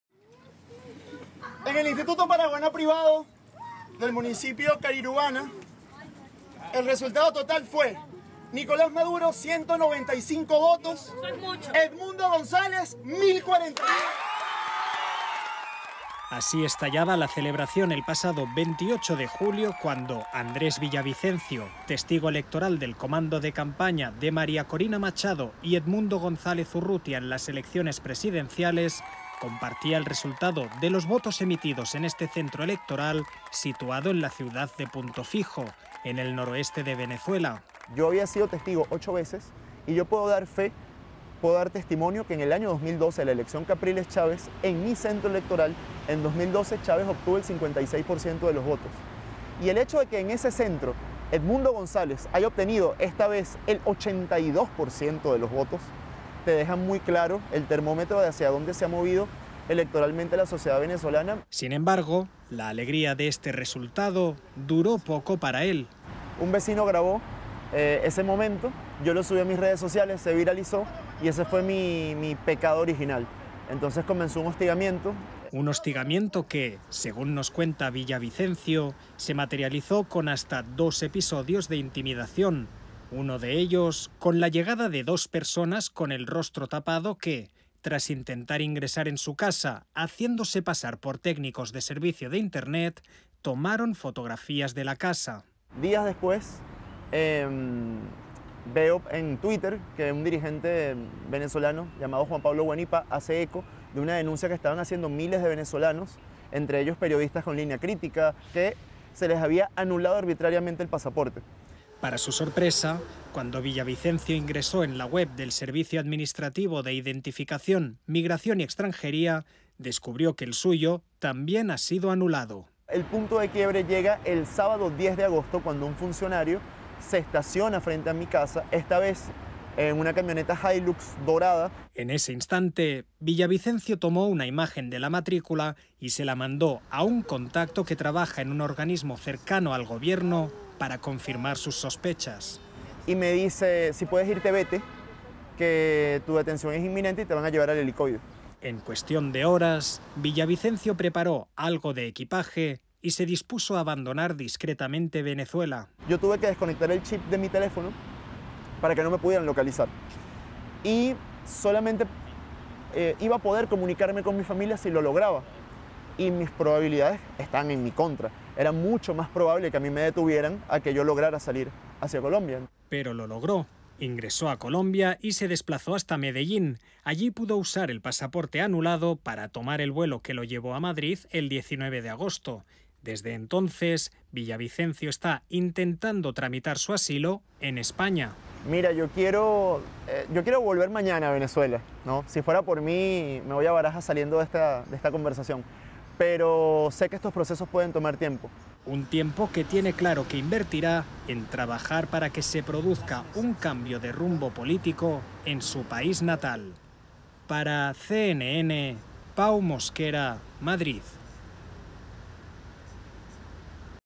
Joven venezolano tuvo que abandonar el país tras ser señalado por su rol en la mesa de votación
La crisis política que vive Venezuela está llevando a más de uno a tomar decisiones drásticas y abandonar el país. Entre estos emigrantes se cuentan activistas políticos y opositores al Gobierno, quienes deciden tomar otro rumbo en el exterior ante el miedo de posibles represalias. Ese es el caso de Andrés Villavicencio, un joven que desde el 19 de agosto se encuentra en España tras ser señalado por su rol en una mesa de votación.